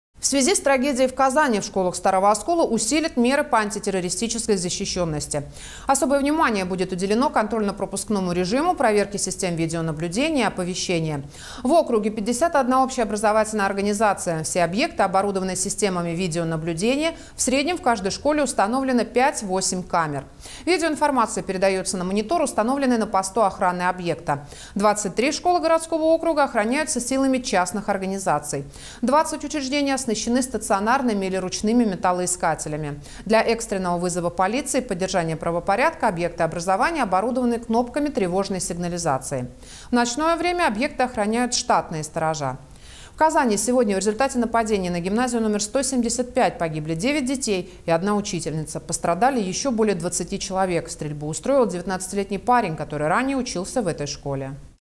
Чтобы не случился старооскольский "колумбайн"
В связи с трагедией в Казани, в школах Старого Оскола усилят меры по антитеррористической защищенности. Особое внимание будет уделено контрольно-пропускному режиму, проверке систем видеонаблюдения и оповещения.